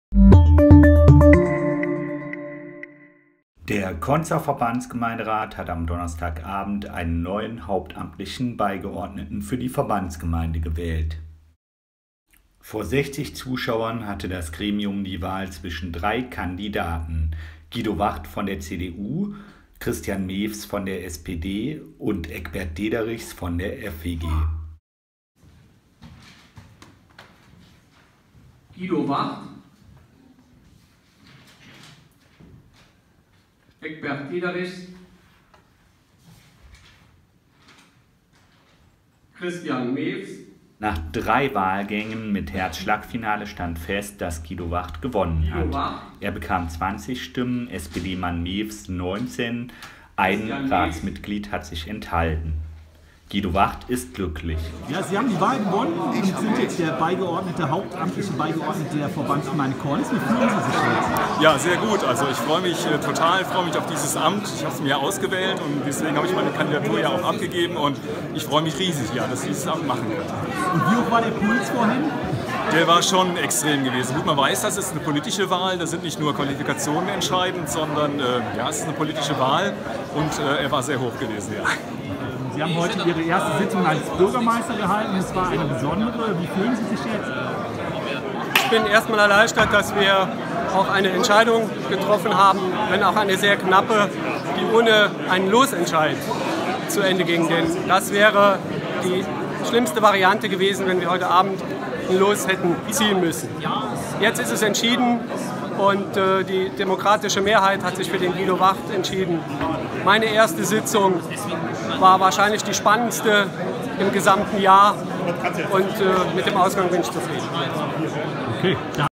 Guido Wacht wird zum neuen Beigeordneten gewählt
Der Verbandsgemeinderat Konz hat am 11. Januar 2018 Guido Wacht (CDU) zum neuen hauptamtlichen Beigeordneten der Verbandsgemeinde gewählt. Er bekam eine Stimme mehr als sein schärfster Konkurrent Christian Mews (SPD). Egbert Dederichs (FWG) musste nach zwei Wahlgängen passen.